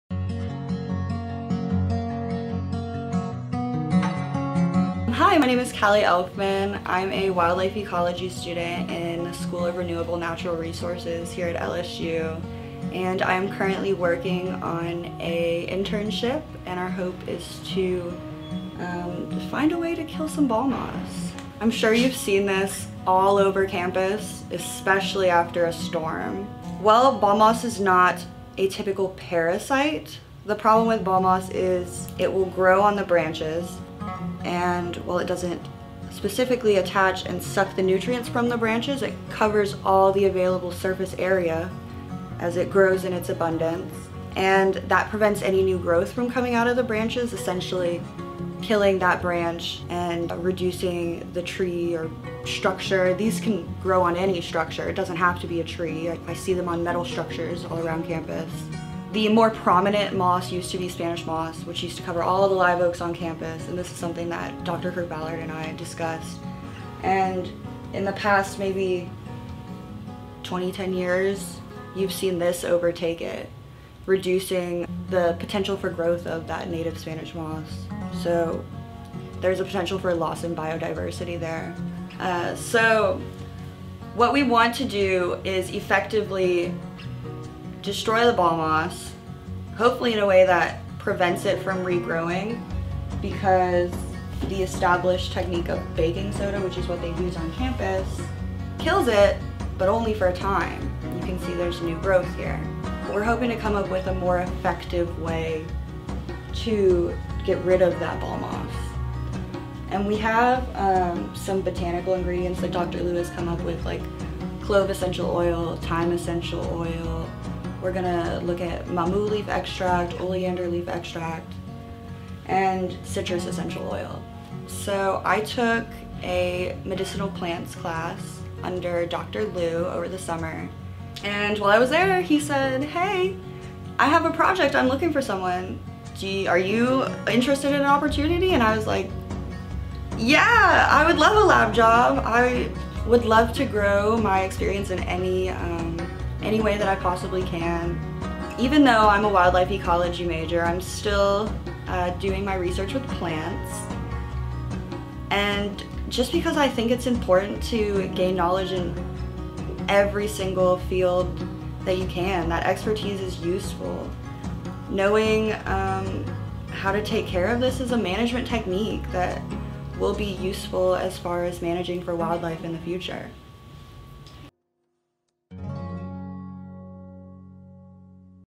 What is ball moss and how is killing other plants? (Undergrad Research)
Kali Elftmann is a senior majoring in natural resource and ecology management.

Her undergraduate research project is titled “Using Botanical Ingredients to Control Ball Moss on Ornamental Plants.” She is trying to figure out how to control the growth of ball moss on LSU's campus🍃

Ball moss is notorious around campus for accumulating on plants and buildings. Find out why ball moss is such a big problem and how it can slowly kill other plants🌳

She is working under the direction of Zhijun Liu, a professor in the School of Renewable Natural Resources, and Heather Kirk-Ballard, a professor in the School of Plant, Environmental and Soil Sciences.